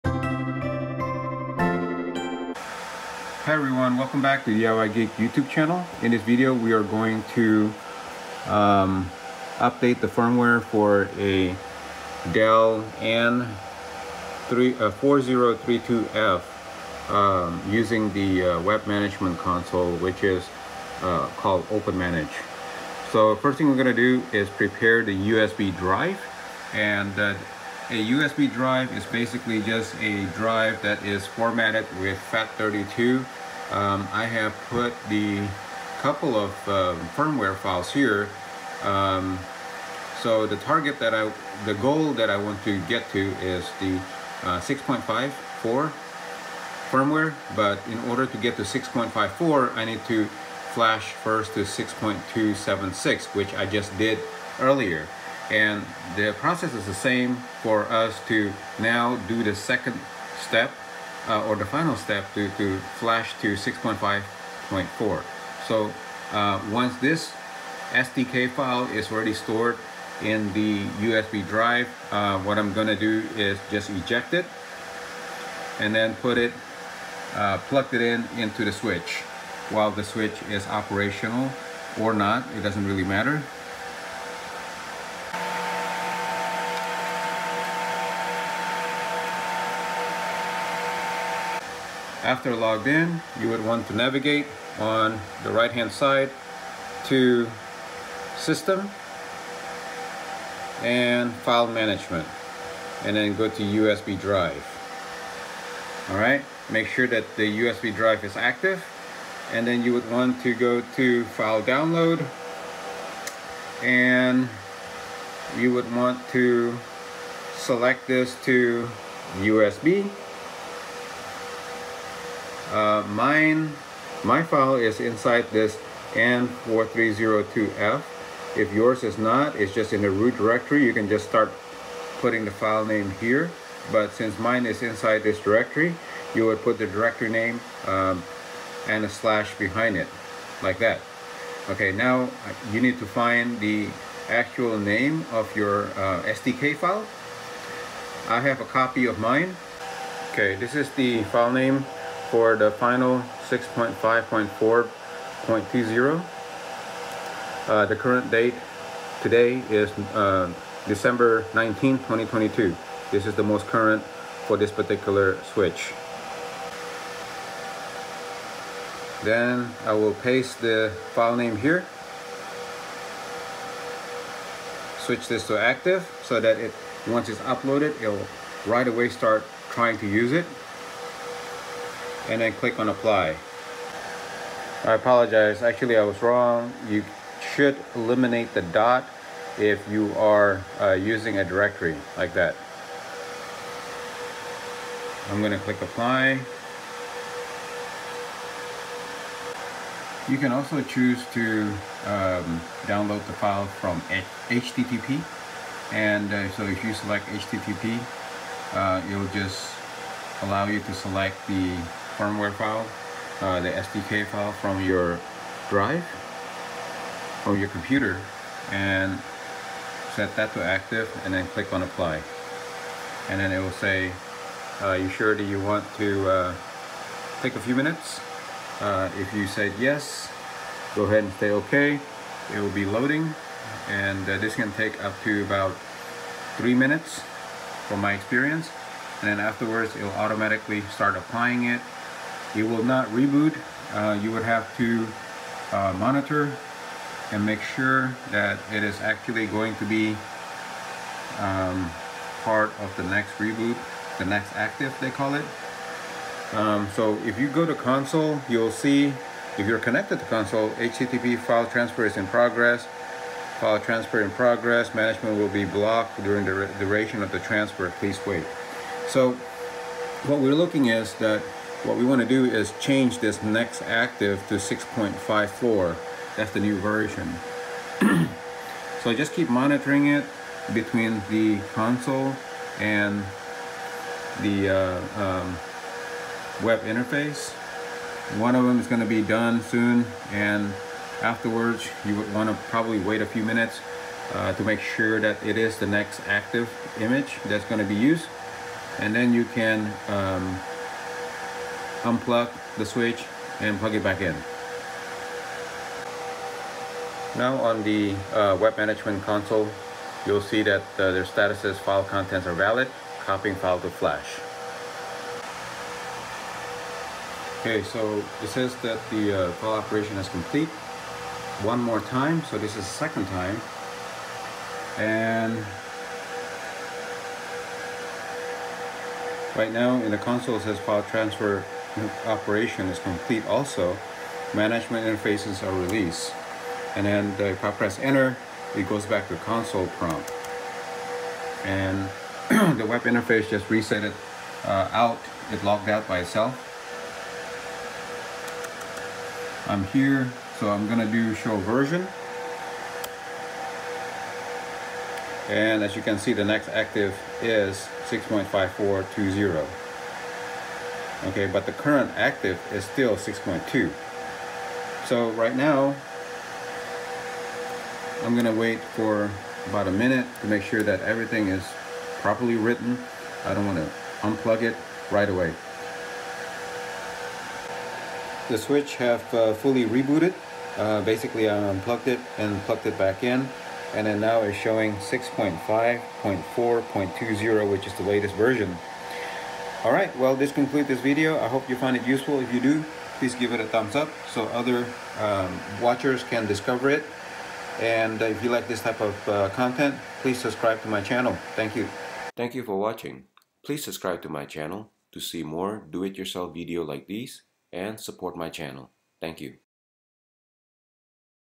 HOW TO UPDATE FIRMWARE on DELL N4032/N4032F/N-SERIES NETWORK SWITCH
How to update firmware for DELL N4032/N4032F/N-SERIES NETWORK SWITCH. Dell N4032F is a great 10Gbe network switch. It is selling for about $300 from eBay for a used one.